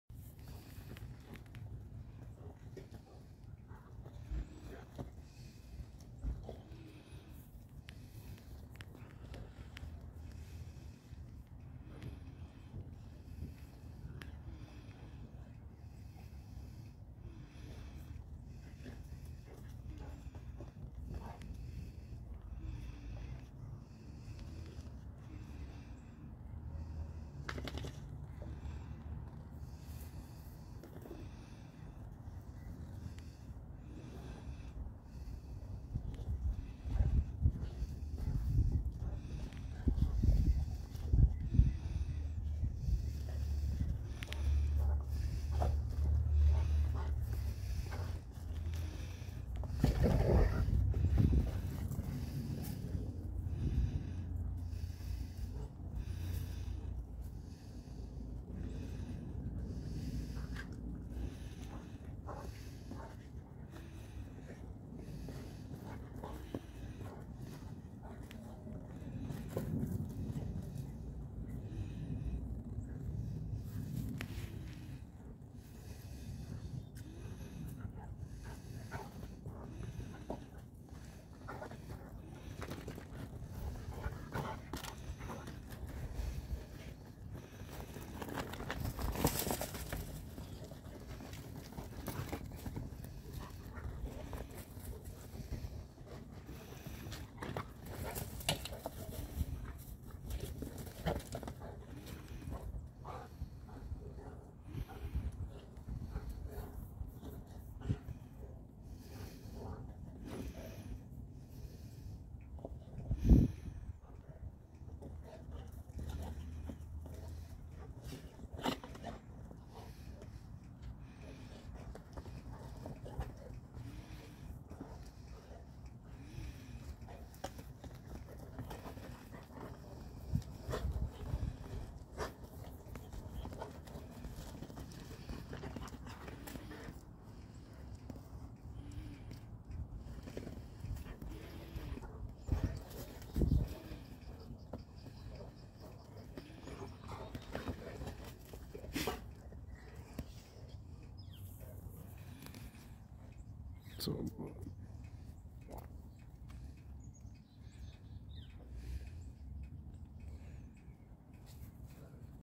Boerboel & Serbian Defense Dog
Attie... 3 year old, Female Brindle Boerboel

The Boerboel is the shorter stockier dog

O.C.D Lagertha, 1 year old, Female Brindle, Serbian Defense Dog

Leland: 9.5 years old, Male American Bully